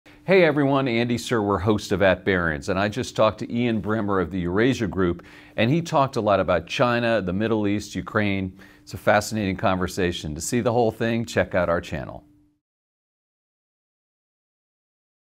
Ian Bremmer joined At Barron's to discuss China, the Middle East, Ukraine, and more
Barron's is the world's premier investing publication since 1921.